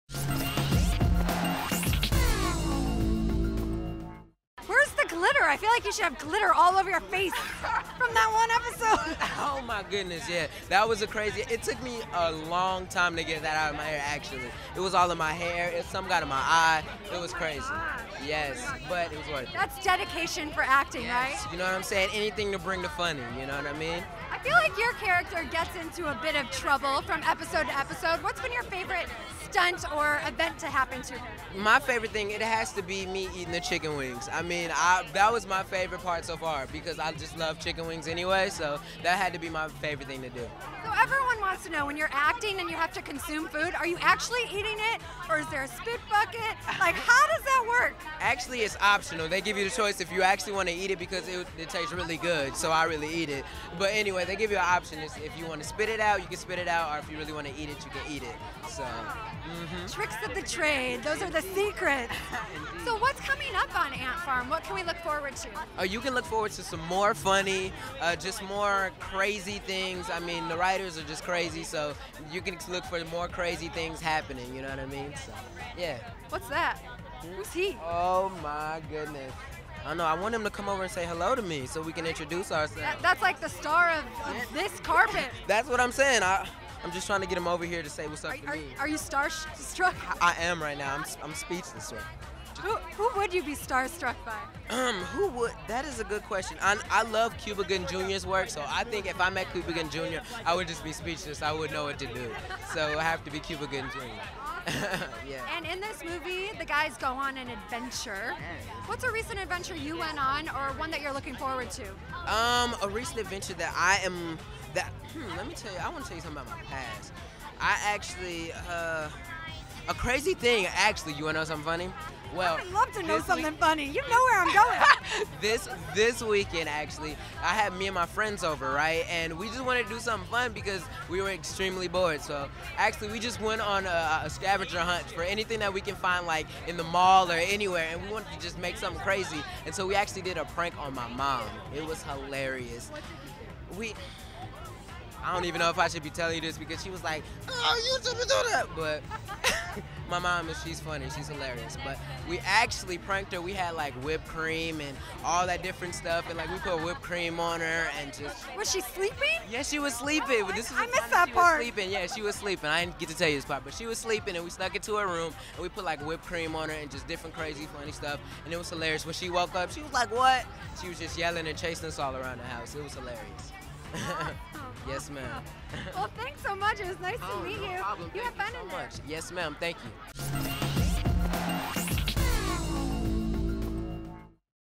Carlon Jeffery At 'Phineas & Ferb' Premiere
Dana Ward interviewed Carlon Jeffery at the 'Phineas & Ferb' premiere.  Carlon plays Cameron on 'A.N.T. Farm' and dishes on his favorite stunts and scenes on the show.